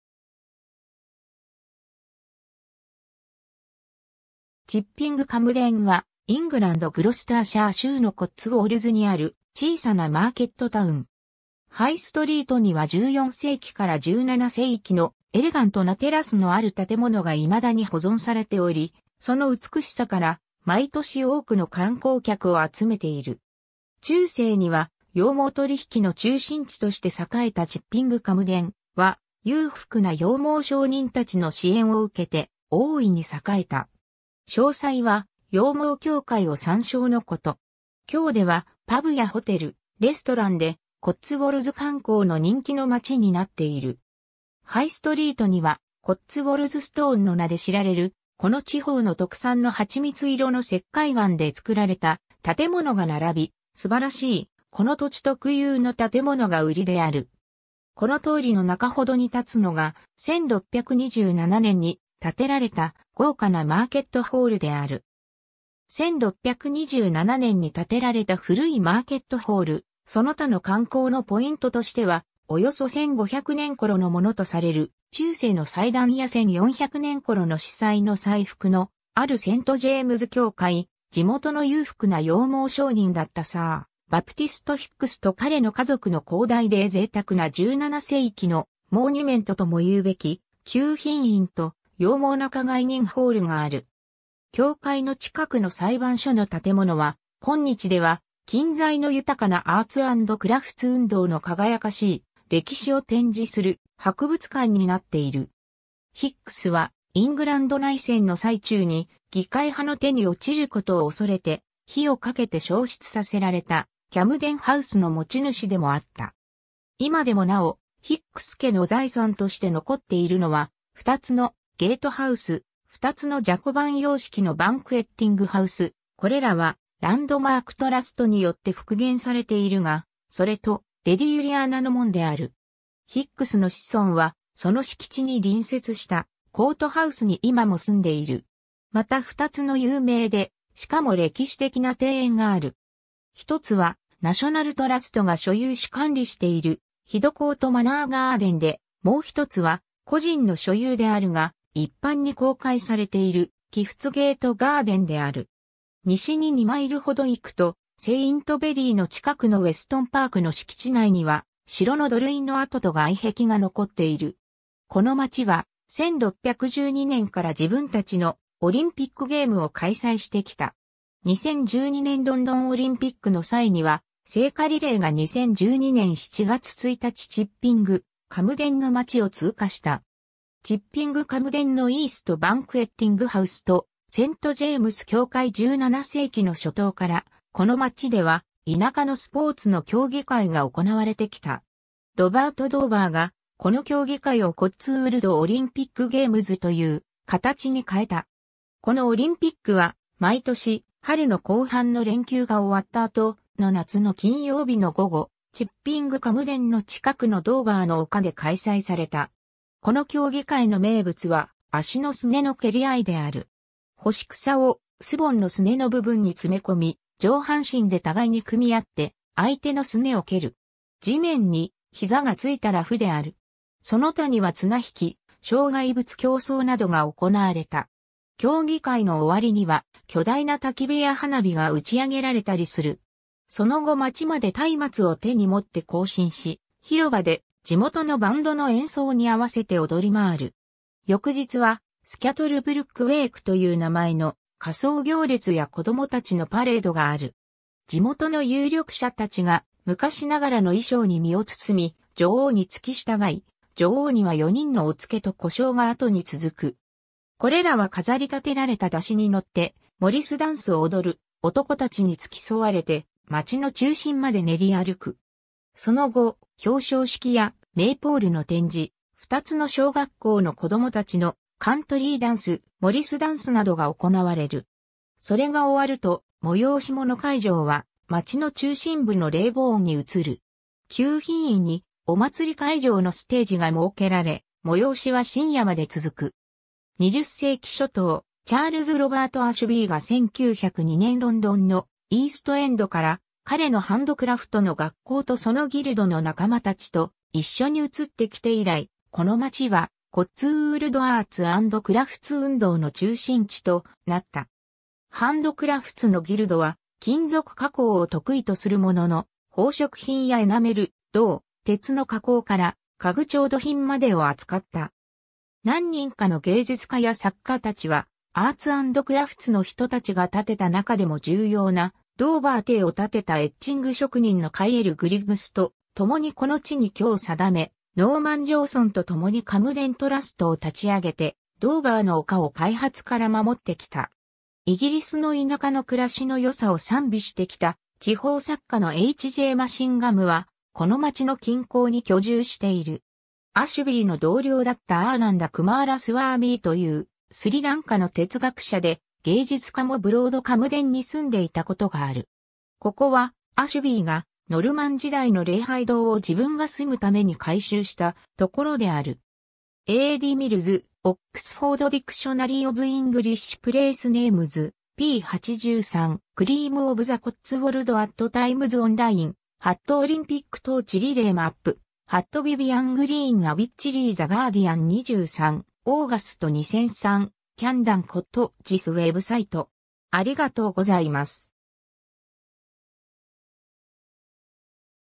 チッピング・カムデン
コッツウォルド_(ディストリクト)
チッピング・カムデン(Chipping Campden)はイングランド・グロスターシャー州のコッツウォルズにある小さなマーケットタウン。
ハイストリートには14世紀から17世紀のエレガントなテラスのある建物が未だに保存されており、その美しさから毎年多くの観光客を集めている。
（チッピングは、古英語の cēpingから由来し、「市場」、「市の立つ場所」の意味である。同様の由来の地名には、チッピング・ノートン、チッピング・サドベリー、チッピング・ウェイコムがある。チッピング・ウェイコムは、現在ではハイ・ウェイコムの名に変わった。） 中世には羊毛取引の中心地として栄えたチッピング・カムデンは、裕福な羊毛商人たちの支援を受けて大いに栄えた。
詳細は、羊毛教会（ en:wool church）を参照のこと。
今日では、パブやホテル、レストランで、コッツウォルズ観光の人気の町になっている。
 ハイストリートには、コッツウォルズストーンの名で知られるこの地方の特産の蜂蜜色の石灰岩で造られた建物が並び、素晴らしいこの土地特有の建物が売りである。
この通りの中程に立つのが、1627年に建てられた豪華なマーケットホールである。
 1627年に建てられた古いマーケットホール その他の観光のポイントとしては、およそ1,500年頃のものとされる中世の祭壇や1,400年頃の司祭の祭服（コープ）のあるセント・ジェームズ教会、地元の裕福な羊毛商人だったサー・バプティスト・ヒックスと彼の家族の広大で贅沢な17世紀のモニュメントともいうべき、救貧院と羊毛仲買人ホールがある。
教会の近くの裁判所の建物は、今日では近在の豊かなアーツ・アンド・クラフツ運動の輝かしい歴史を展示する博物館に成っている。
（以下を参照のこと。） ヒックスは、イングランド内戦 の最中に議会派の手に落ちることを恐れて、火をかけて焼失させられたキャムデンハウスの持ち主でも合った。
今でもなおヒックス家の財産として残っているのは、二つのゲートハウス、二つのジャコバン様式のバンクェッティングハウス、これらはランドマーク・トラストによって復元されているが、それとレディ・ユリアナの門である。
ヒックスの子孫は、その敷地に隣接したコートハウスに今も住んでいる。
 また二つの有名で、しかも歴史的な庭園がある。
一つは、ナショナル・トラストが所有し管理しているヒドコート・マナー・ガーデンで、もうひとつは個人の所有であるが、一般に公開されているキフツゲート・ガーデンである。
 西に2マイルほど行くと、セイントベリーの近くのウェストンパークの敷地内には、城の土塁の跡と外壁が残っている。
 この町は、1612年から自分たちのオリンピックゲームを開催してきた。
2012年ロンドンオリンピックの際には、聖火リレーが2012年7月1日チッピング・カムデンの町を通過した。
 チッピング・カムデンのイースト・バンクェッティングハウスとセントジェームス教会 17世紀の初頭から、この町では、田舎のスポーツの競技会が行われてきた。
ロバート・ドーバーが、この競技会をコッツウォールド・オリンピックゲームズというかたちに変えた。
このオリンピックは、毎年、春の後半の連休が終わった後、（通常、5月末か6月初め）の夏の金曜日の午後、チッビング・カムデンの近くのドーバーの丘（ロバート・ドーバーの所有地だったところ）で開催された。
この競技会の名物は、足のすねの蹴り合いである。
干し草をスボンのすねの部分に詰め込み、上半身で互いに組み合って相手のすねをける。
地面に膝がついたら負である。
その他には綱引き、障害物競走などが行われた。
 競技会の終わりには、巨大な焚き火や花火が打ち上げられたりする。
その後町まで松明を手に持って行進し、広場で地元のバンドの演奏に合わせて踊りまわる。
翌日は、スキャトルブルック・ウェイク(The Scuttlebrook Wake)という名前の、仮装行列や子どもたちのパレードがある。
地元の有力者たちが昔ながらの衣装に身を包み、女王につき従い、女王には4人のお付と小姓が後に続く。
これらは飾り立てられた山車に乗って、モリスダンスを踊る男たちに付き添われて町の中心まで練り歩く。
その後、表彰式やメイポールの展示、二つの小学校の子どもたちのカントリーダンス、モリスダンスなどが行われる。
それが終わると、催し物会場は、町の中心部のレイボーンに移る。
救貧院にお祭り会場のステ...